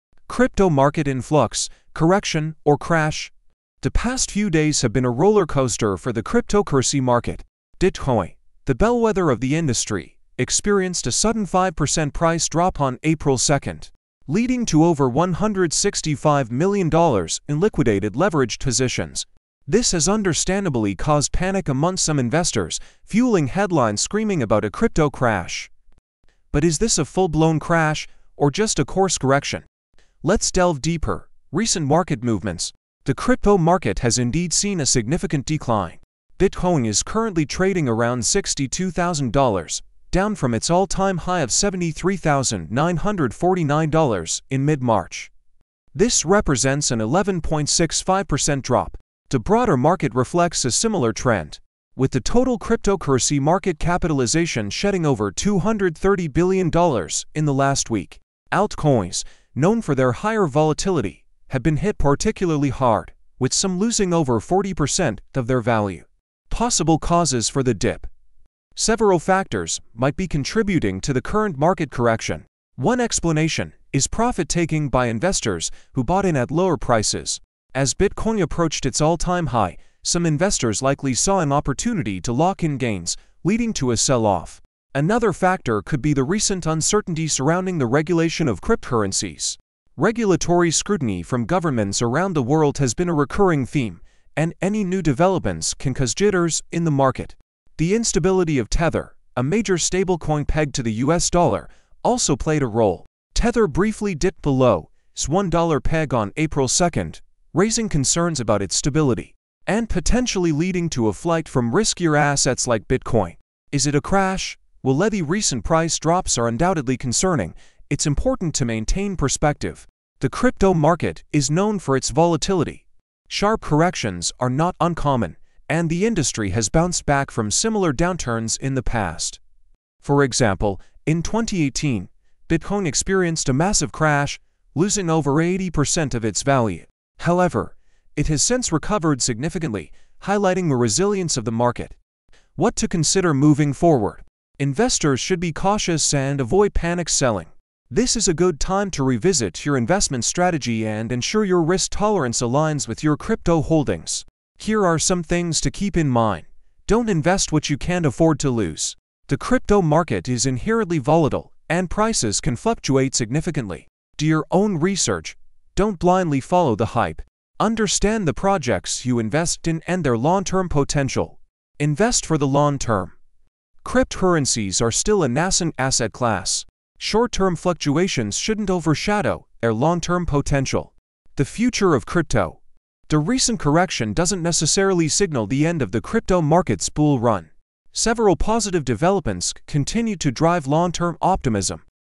IS THE CRYPTO MARKET CRASHING! BREAKING CRYPTO NEWS!cryptocurrency
Crypt Market in Flux: Correction or Crash?

The past few days have been a rollercoaster for the cryptocurrency market. Bitcoin (BTC), the bellwether of the industry, experienced a sudden 5% price drop on April 2nd, leading to over $165 million in liquidated leveraged positions. This has understandably caused panic amongst some investors, fueling headlines screaming about a crypto crash.

But is this a full-blown crash, or just a course correction? Let's delve deeper.

**Recent Market Movements**

The crypto market has indeed seen a significant decline. Bitcoin is currently trading around $62,000, down from its all-time high of $73,949 in mid-March. This represents an 11.65% drop. The broader market reflects a similar trend, with the total cryptocurrency market capitalization shedding over $230 billion in the last week. Altcoins, known for their higher volatility, have been hit particularly hard, with some losing over 40% of their value.

**Possible Causes for the Dip**

Several factors might be contributing to the current market correction. One explanation is profit-taking by investors who bought in at lower prices. As Bitcoin approached its all-time high, some investors likely saw an opportunity to lock in gains, leading to a sell-off.

Another factor could be the recent uncertainty surrounding the regulation of cryptocurrencies. Regulatory scrutiny from governments around the world has been a recurring theme, and any new developments can cause jitters in the market.

The instability of Tether (USDT), a major stablecoin pegged to the US dollar, also played a role. Tether briefly dipped below its $1 peg on April 2nd, raising concerns about its stability and potentially leading to a flight from riskier assets like Bitcoin.

**Is it a Crash?**

While the recent price drops are undoubtedly concerning, it's important to maintain perspective. The crypto market is known for its volatility.  Sharp corrections are not uncommon, and the industry has bounced back from similar downturns in the past.

For example, in 2018, Bitcoin experienced a massive crash, losing over 80% of its value. However, it has since recovered significantly, highlighting the resilience of the market.

**What to Consider Moving Forward**

Investors should be cautious and avoid panic selling.  This is a good time to revisit your investment strategy and ensure your risk tolerance aligns with your crypto holdings.

Here are some things to keep in mind:

* **Don't invest what you can't afford to lose.** The crypto market is inherently volatile, and prices can fluctuate significantly.
* **Do your own research.**  Don't blindly follow the hype. Understand the projects you invest in and their long-term potential.
* **Invest for the long term.** Cryptocurrencies are still a nascent asset class. Short-term fluctuations shouldn't overshadow their long-term potential.

**The Future of Crypto**

The recent correction doesn't necessarily signal the end of the crypto market's bull run.  Several positive developments continue to drive long-term optimism:

* **Institutional adoption:** More and more financial institutions are entering the crypto space, lending legitimacy to the industry.
* **Technological advancements:**  The underlying blockchain technology is constantly evolving, offering new possibilities for innovation.
* **Growing awareness:** Public awareness and understanding of cryptocurrencies are on the rise, potentially leading to wider adoption.

**Conclusion**

The crypto market is currently experiencing a correction. While this can be unsettling for investors, it's crucial to maintain perspective.  Sharp price drops have happened before, and the industry has a history of bouncing back.

Focus on long-term fundamentals, prioritize responsible investing practices, and remember, volatility is part of the crypto game.